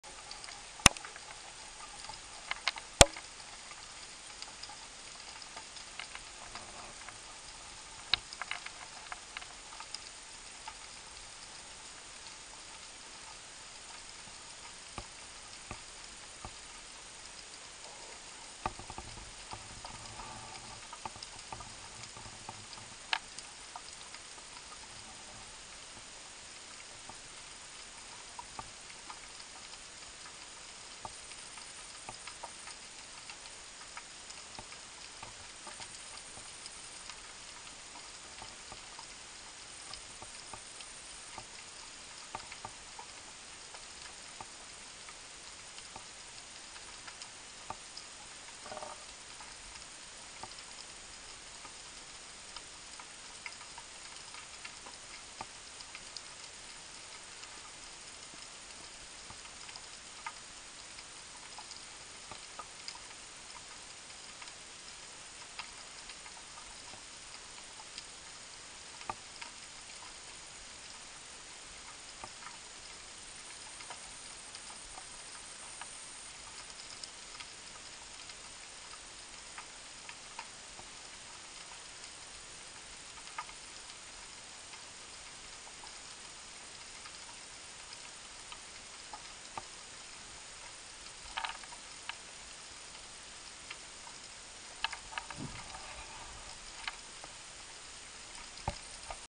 (19.6.1) 황줄깜정이 돌돔
프리다이빙(무호흡잠수)으로 대한민국 수중을 탐사하며 액션캠으로 물고기를 주로 돌돔을 촬영하고 있습니다